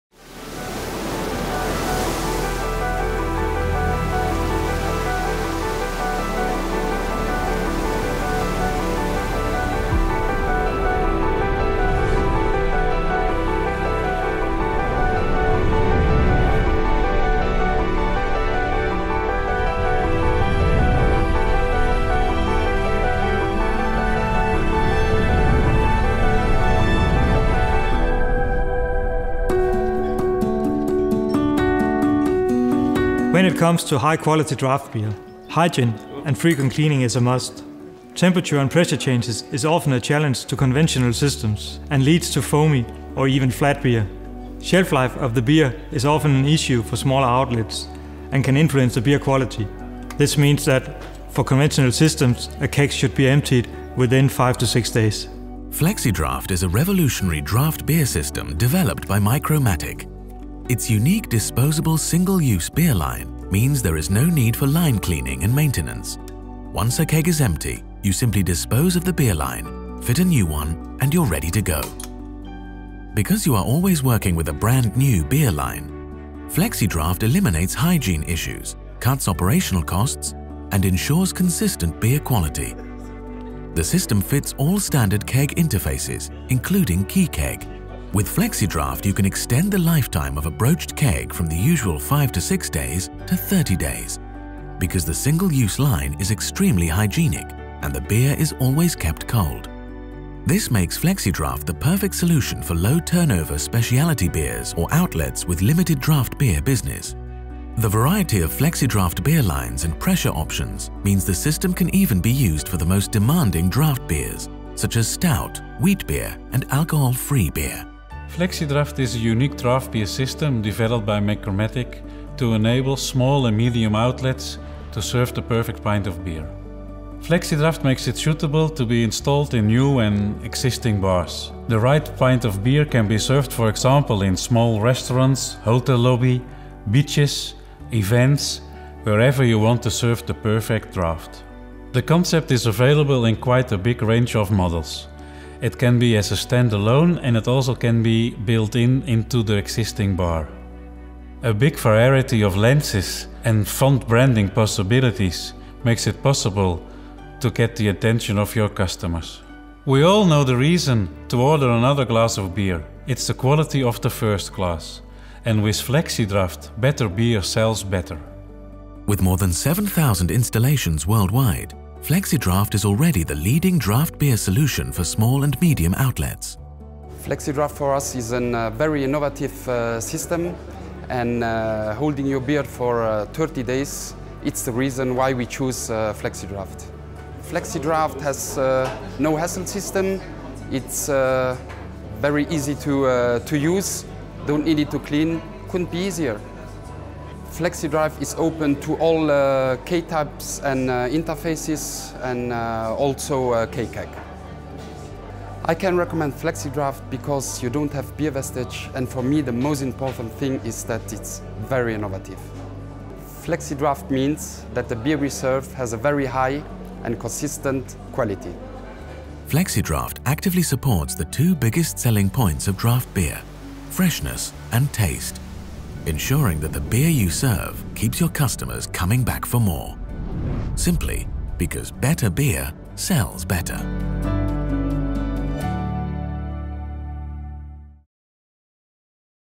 FlexiDraft™ by Micro Matic
FlexiDraft™ is a revolutionary draft beer system. Its unique disposable one-way beer line means no cleaning and no maintenance. It also extends the fresh life of beer in a broached keg from 6 to 30 days.